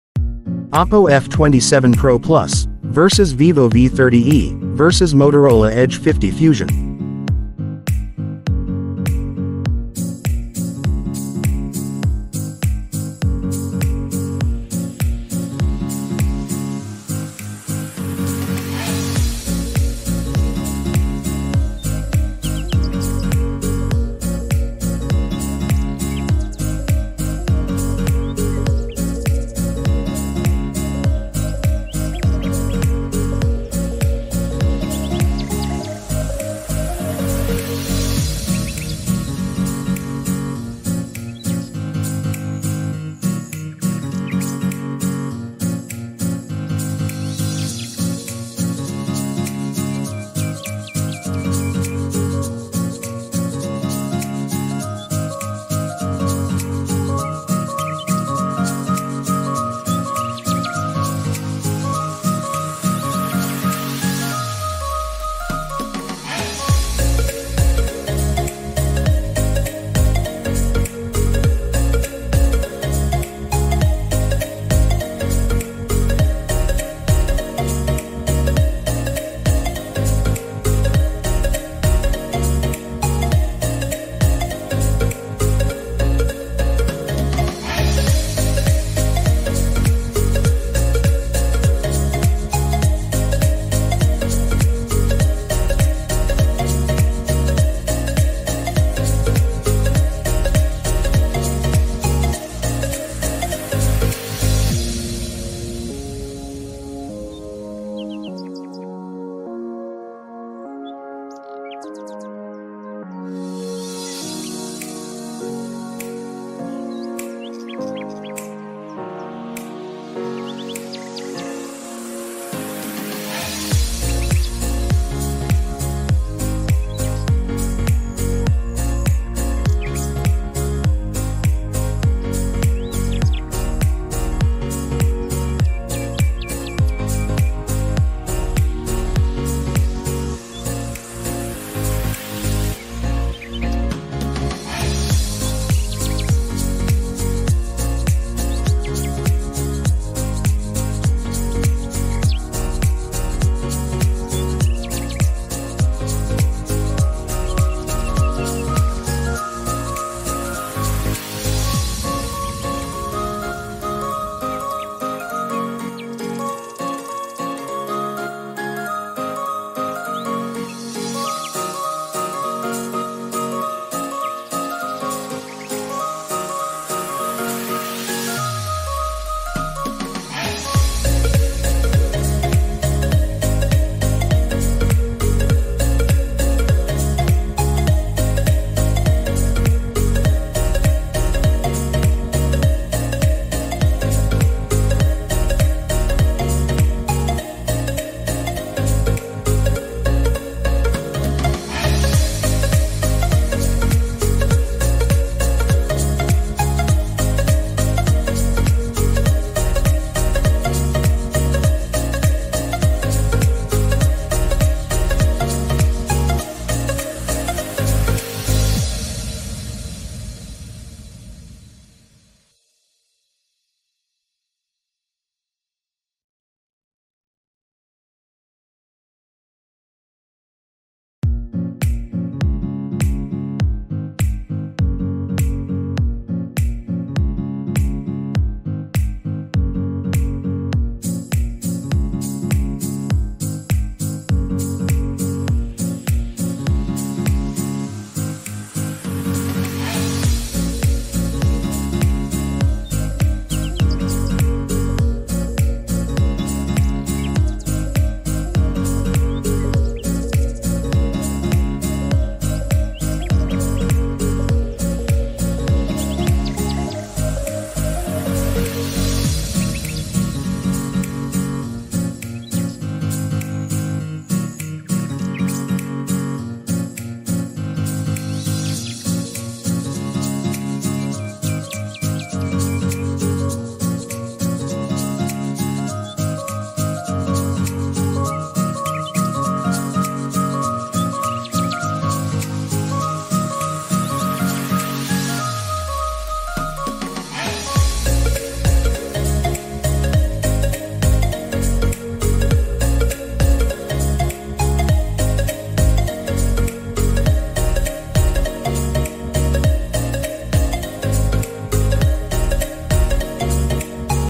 Motorola Edge 50 Fusion vs Oppo F27 Pro Plus vs Vivo V30e || Full Comparison
Motorola Edge 50 Fusion vs Oppo F27 Pro Plus vs Vivo V30e

In this video we will compare :

DISPLAY
SCREEN TYPE
REFRESH RATE
BRIGHTNESS
SCREEN SIZE
BODY RATIO
ASPECT RATIO
RESOLUTION
PPI DENSITY
PROTECTION
BODY FRAME
WATER RESISTENT
SOFTWARE
OPERATING SYSTEM
CHIPSET
PROCESSOR
GPU
STORAGE
RAM
INTERNAL MEMORY
MEMORY CARD
CAMERA
MAIN CAMERA
MAX RECORD
ZOOM CAMERA
SELFIE CAMERA
SOUND
STEREO SPEAKERS
3.5 mm Jack
CONNECTIVITY
NETWORK
FINGERPRINT
BLUETOOTH
USB TYPE
NFC
BATTERY
CHARGING
PRICE